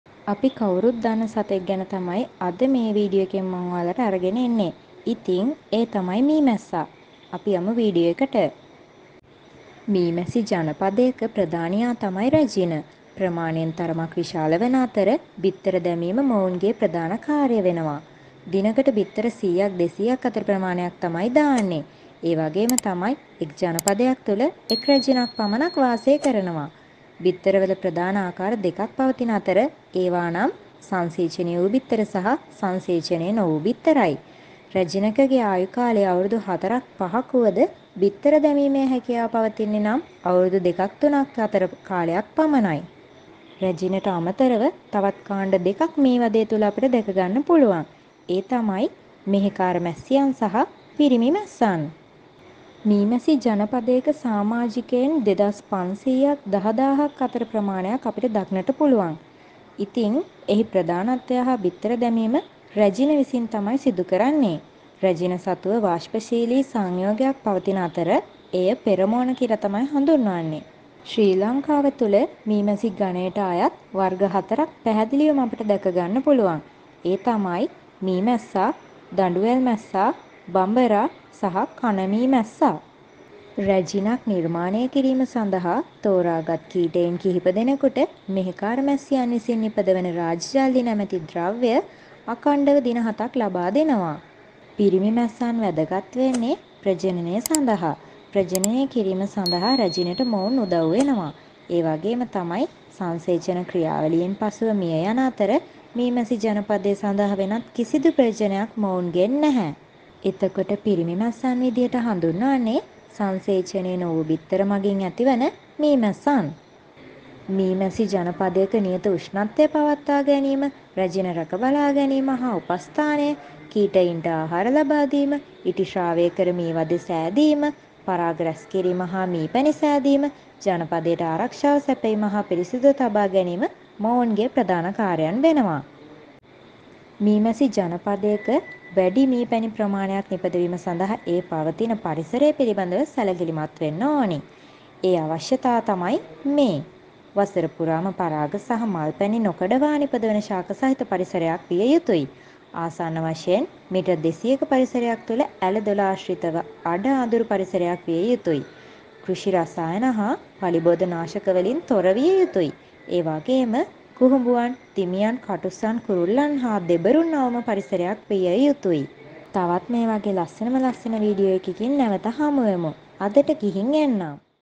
මීමැස්සන් ගැන පුදුම හිතෙන කරුනු කිහිපයක් - Learn these top 10 facts about bees
ඔබ මීමැස්සන් ගැන දැනගත යුතු කරුනු කිහිපයක්

Learn these top 10 facts about bees

About bee queen and all the details